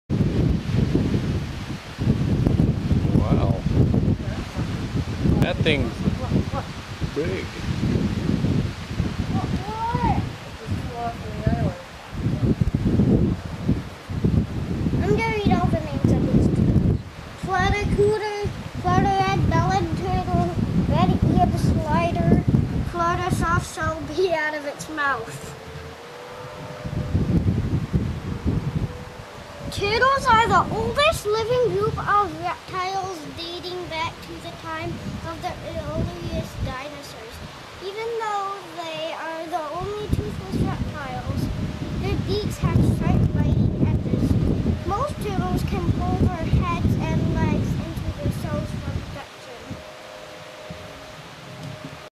Hanging at The Wakodahatchee Wetlands
Hanging at The Wakodahatchee Wetlands @ Delray Beach, FL.  I like this one because Ben's reading so well in it.